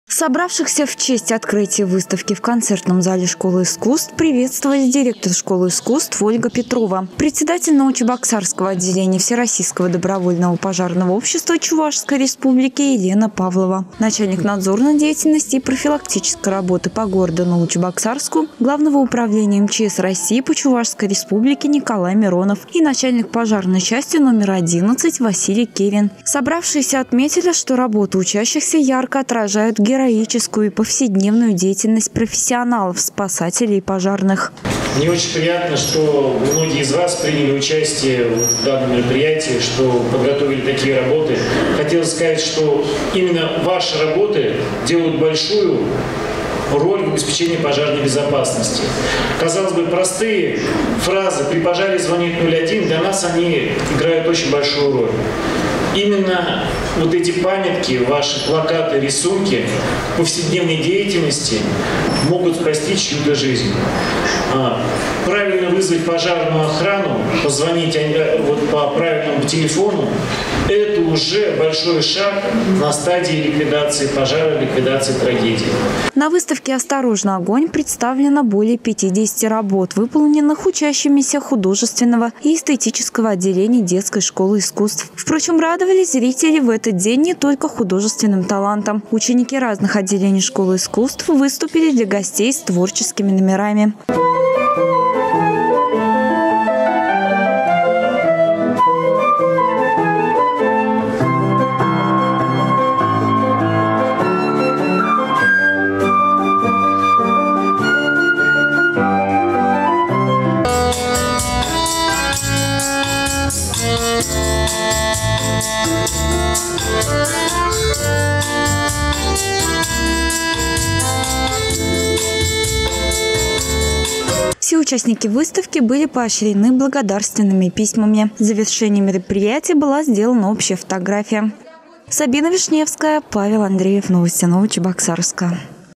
Выставка творческих работ «Осторожно! Огонь!» в Детской школе искусств
В Детской школе искусств города состоялось открытие выставки творческих работ по пожарной безопасности «Осторожно! Огонь!», посвященной 100-летию со дня основания Советской Пожарной охраны.